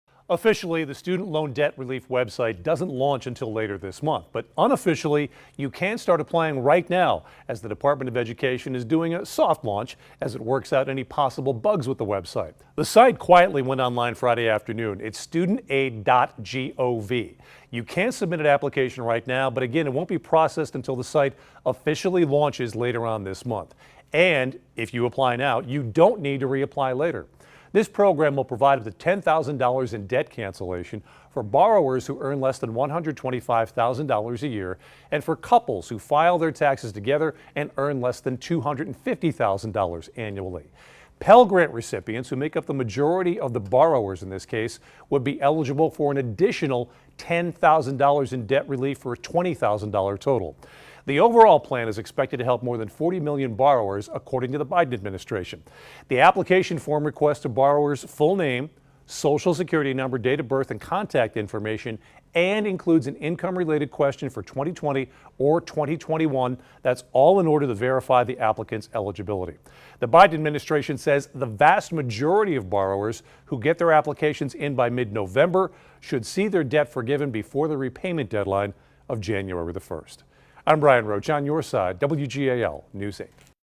Student debt forgiveness application now open
Federal student loan borrowers can apply for up to $20,000 in debt forgiveness.